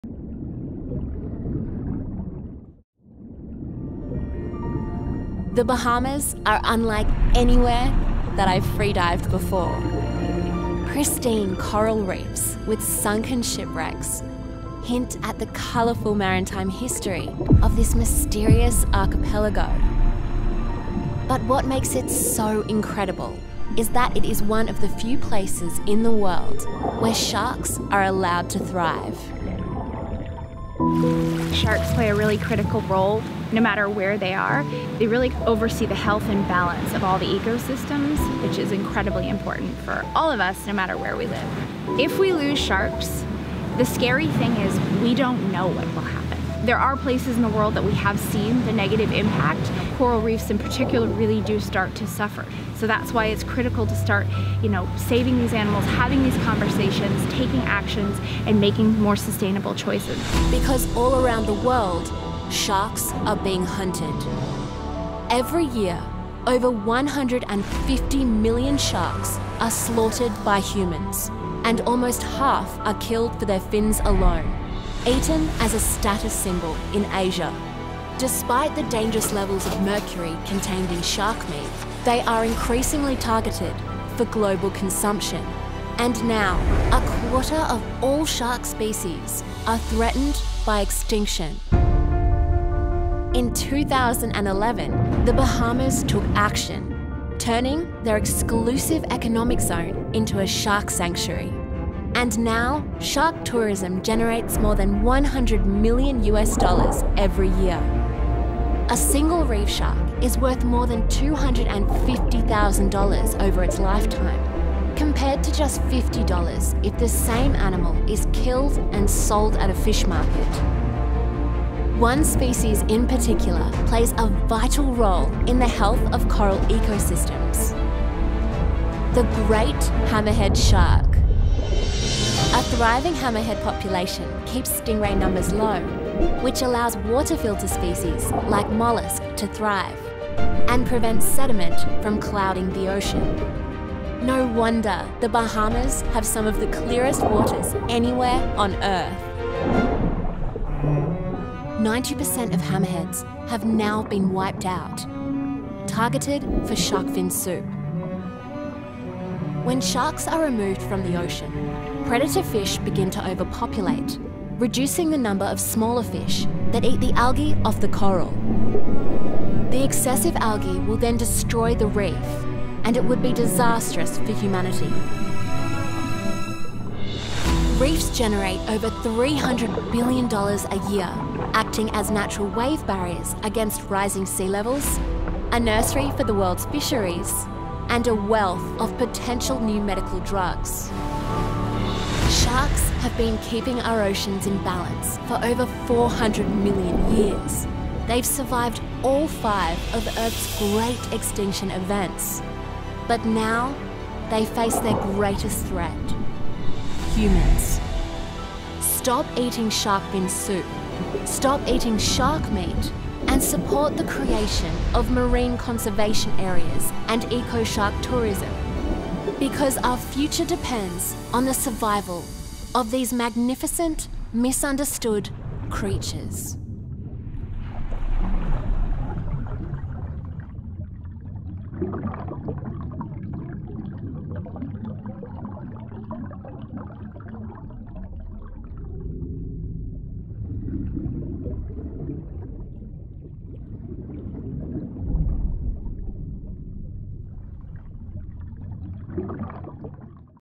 Shark Attack on World Oceans Day | By Photographer & Freediver Julia Wheeler
This World Ocean Day, take a dive into the fascinating world of the ocean’s most misunderstood apex predators with freediver and photographer Julia Wheeler on a single breath. Filmed on the 5D Mark IV.

Raising awareness of destructive, unsustainable fishing that is altering the complex balance of the oceans, Julia exposes that the 60-270 million sharks killed every year is driving them to extinction and threatening the future of humanity.

Freediver: @iamjuliawheeler
Colouist: Matt Campbell
Sound: Luke Brodie